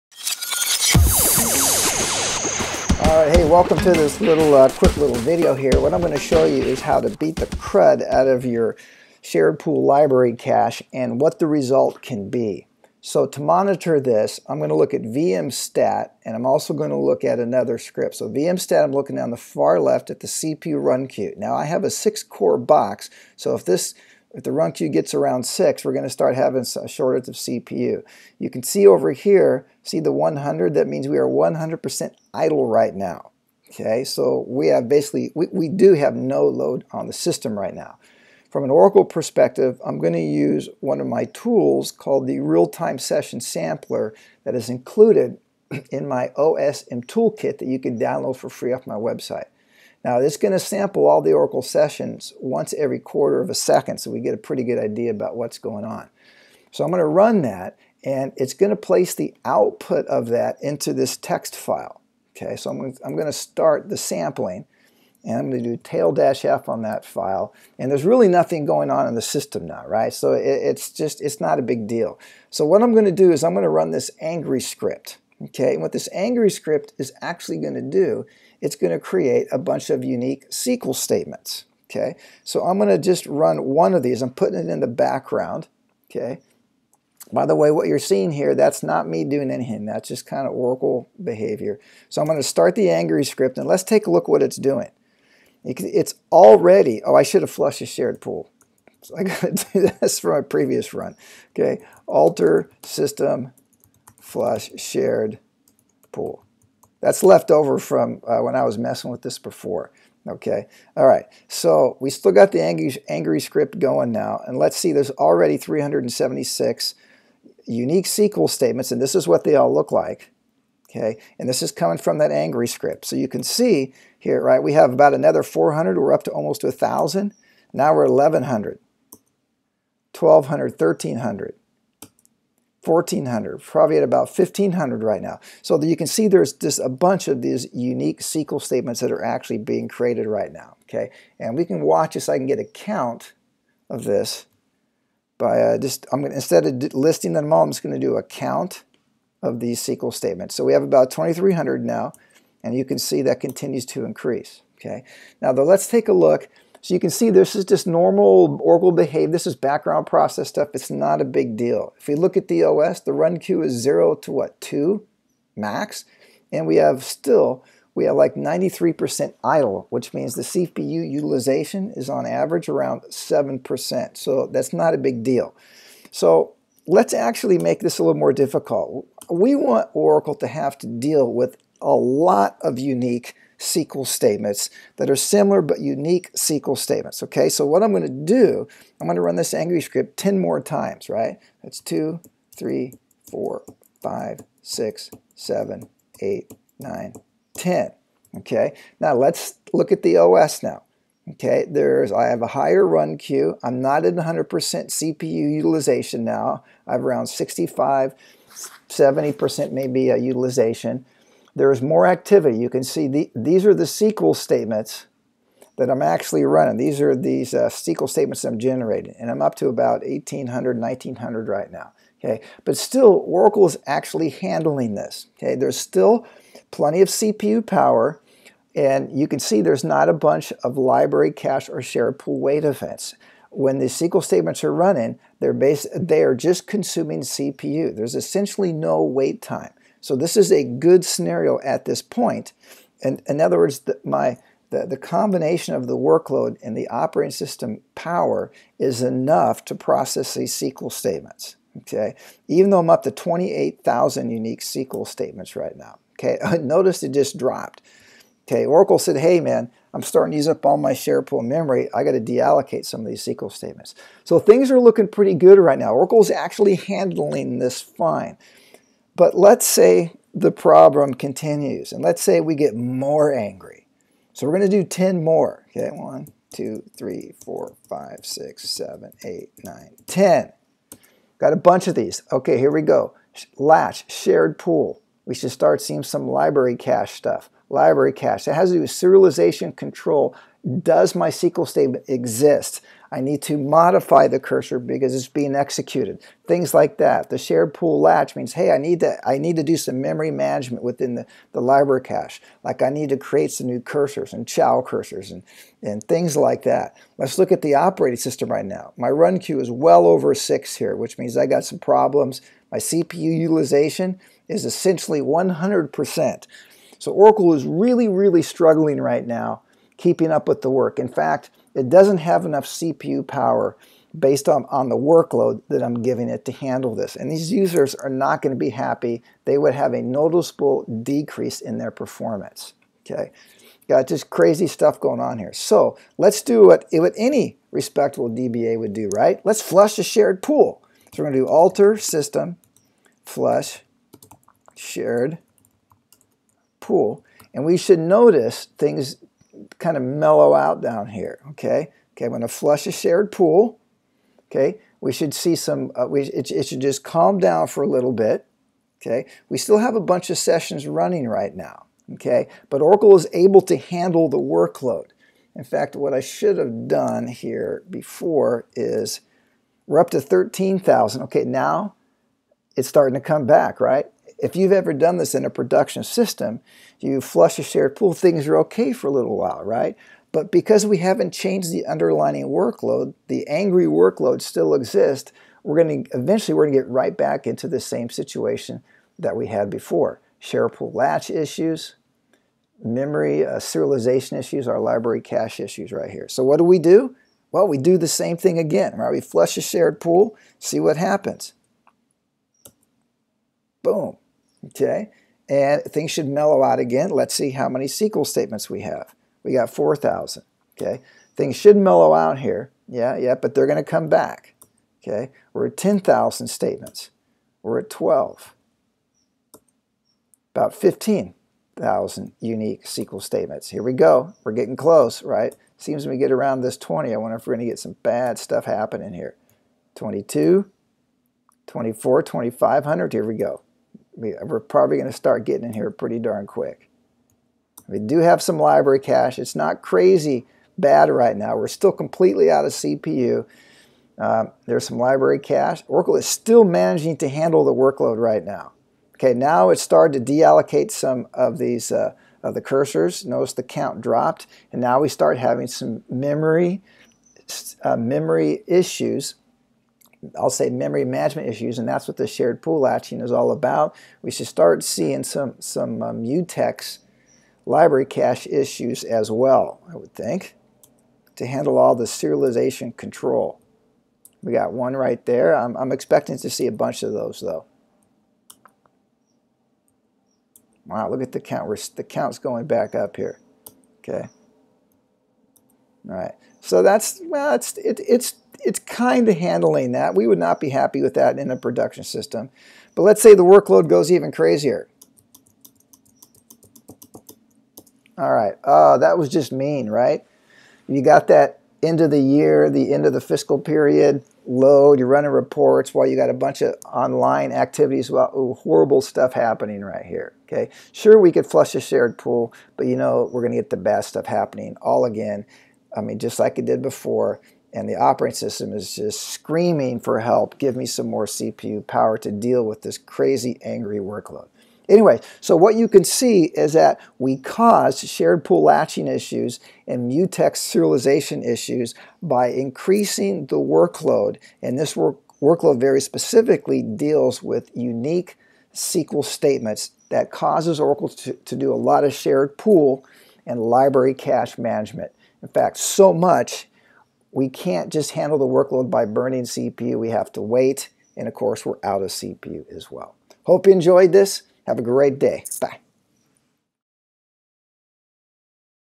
How an intense and diverse SQL statement workload can cause Oracle parsing problems
Make your library cache angry! This video is about how an intense and diverse SQL statement workload can cause Oracle parsing problems.

There are many reasons for Oracle parsing related performance issues and many ways to identify them. So, I am narrowing the scope by focusing on one possible cause, that is, an intense and diverse SQL statement workload. I will use Oracle's wait interface to identify the root cause.

Sound dry and boring? To make this a little more interesting and a whole lot more fun, I'm going to approach this from a non-traditional, almost backward perspective. After explaining some of the Oracle internals, I'm going to create a parsing intensive workload and then watch what happens... in real time. The educational impact is so powerful, I recorded a video